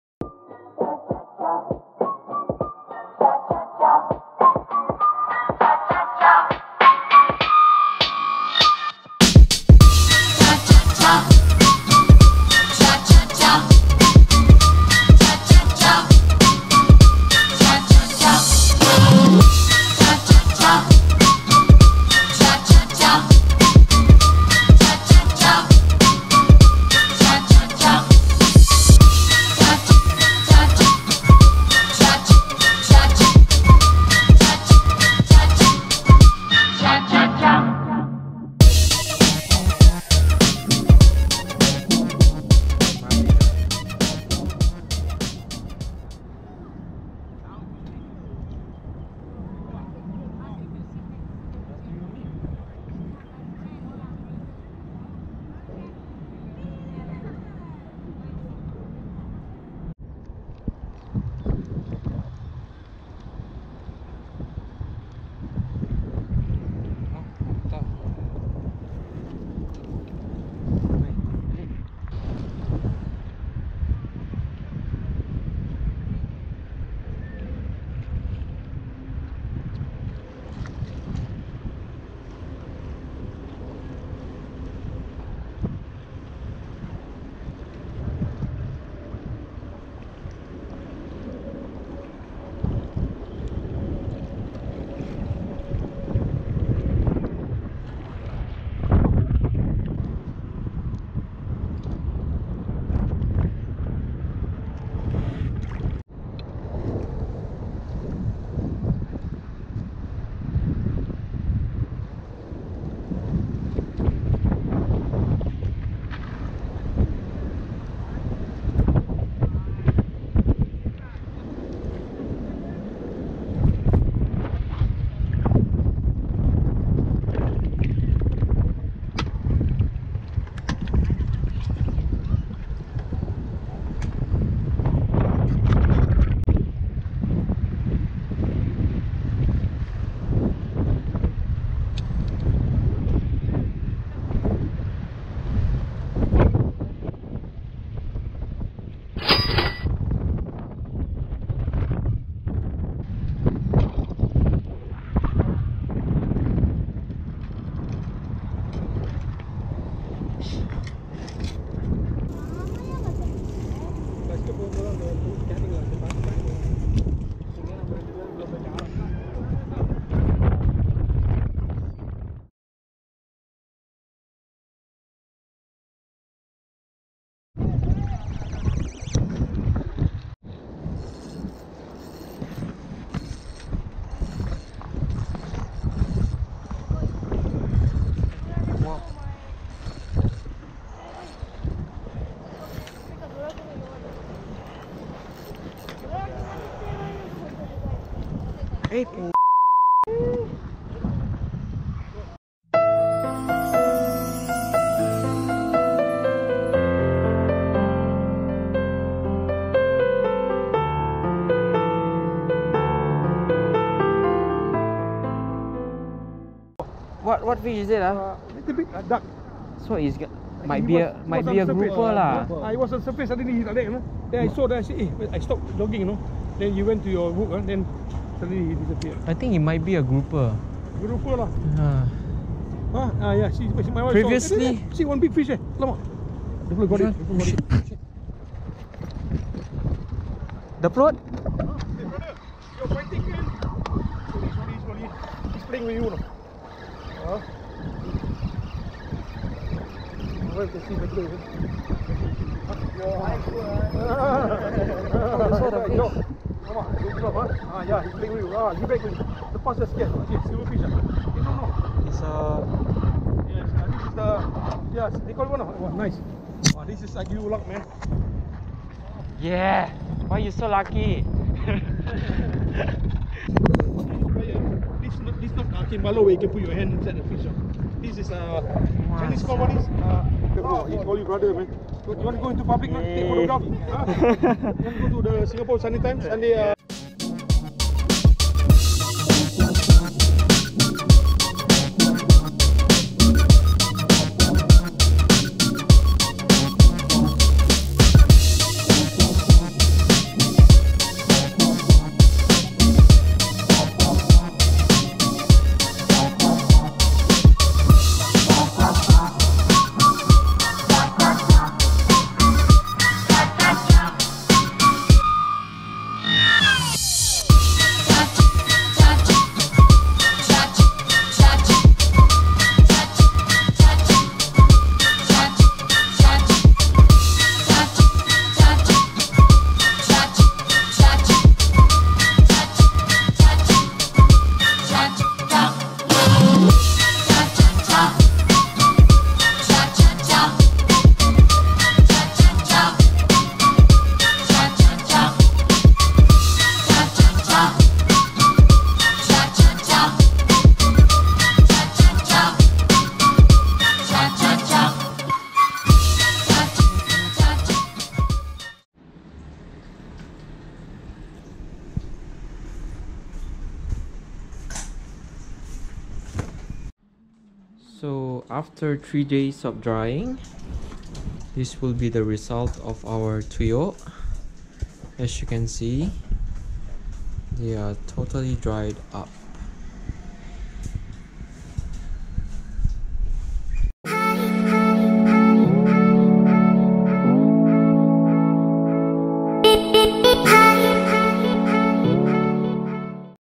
Bedok Jetty Fishing: How to make TUYO
Fishlog 2: In this Fishlog we visited one of the most famous saltwater fishing spot in Singapore “Bedok Jetty”. We will teach you guys how to make TUYO (dried salted tamban). This particular dried fish is well known in the island of Philippines. Enjoy the video and learn how to make tuyo.